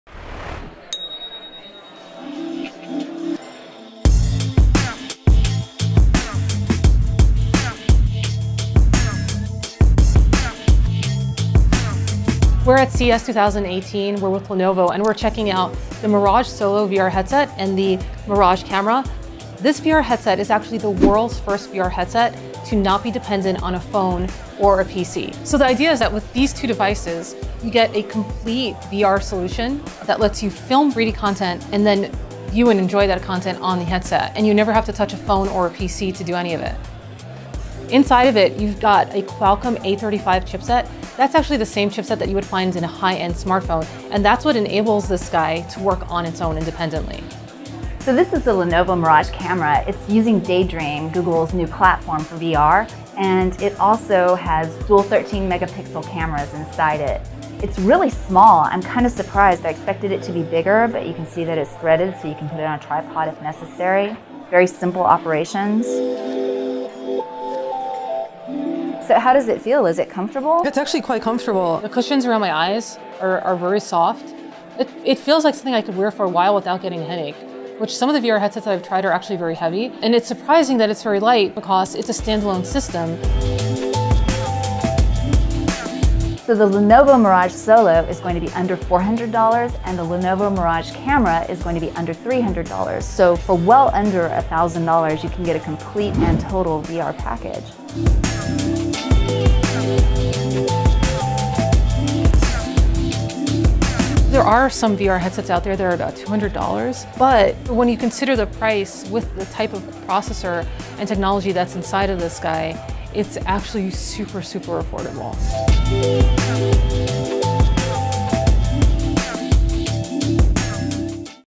Lenovo's Mirage Solo VR Headset and Mirage Camera Hands-on at CES 2018
The Mirage Solo is the world's first VR headset that doesn't require a phone or PC to operate. It's running on Google's Daydream platform.